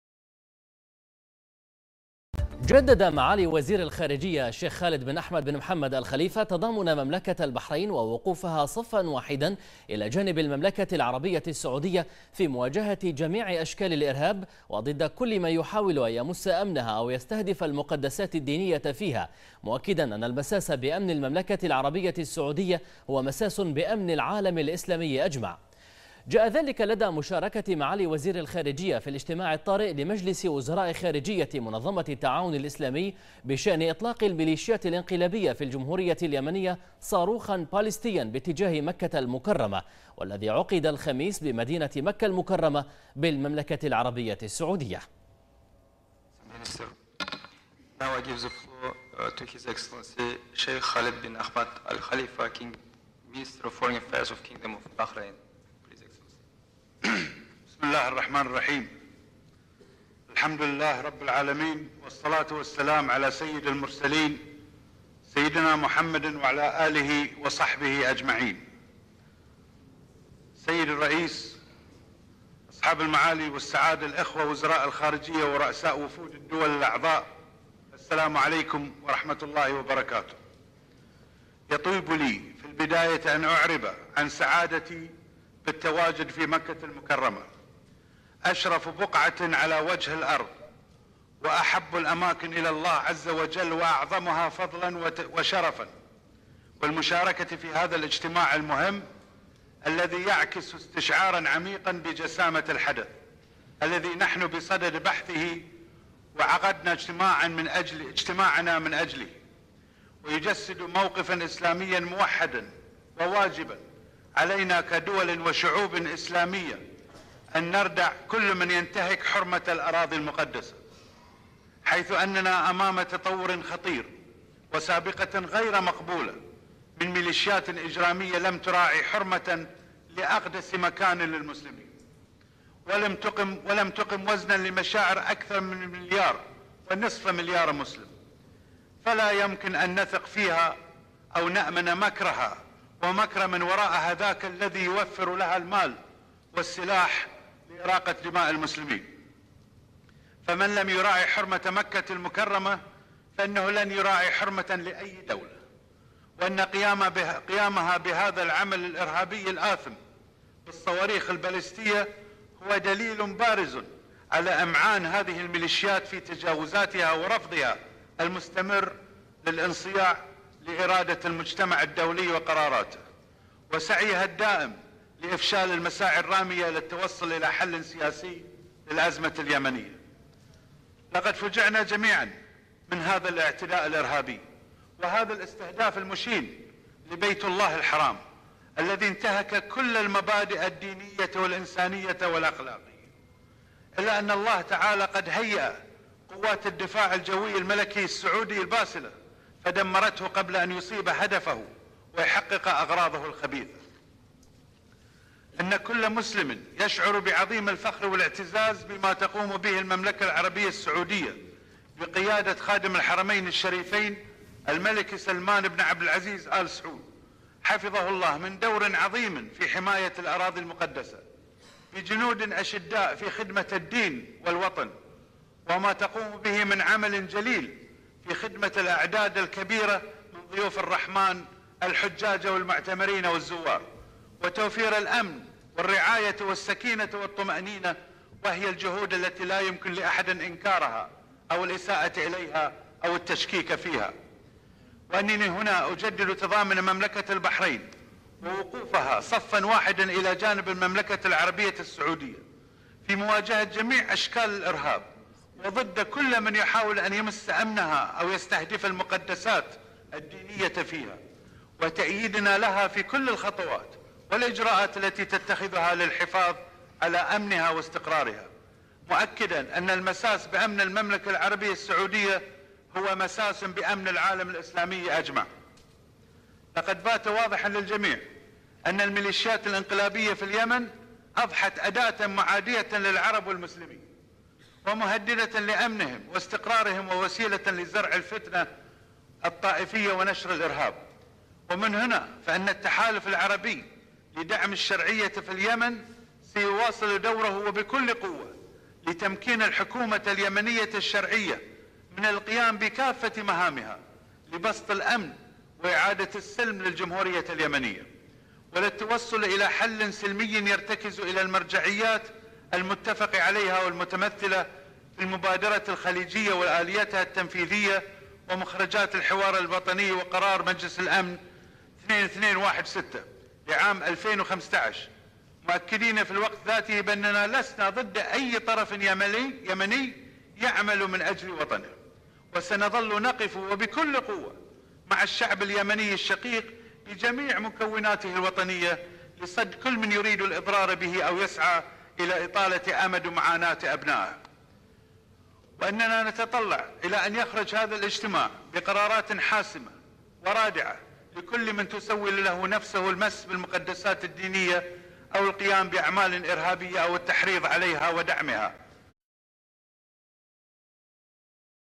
البحرين: معالي وزير الخارجية يشارك في الإجتماع الطارئ لمجلس وزراء خارجية منظمة التعاون الإسلامي
المنامة في 17 نوفمبر/بنا/ شارك معالي الشيخ خالد بن أحمد بن محمد آل خليفة وزير الخارجية في الاجتماع الطارىء لمجلس وزراء خارجية منظمة التعاون الإسلامي بشأن إطلاق الميلشيات الانقلابية في الجمهورية اليمنية صاروخ باليستي باتجاه مكة المكرمة والذي عقد اليوم بمدينة مكة المكرمة بالمملكة العربية السعودية.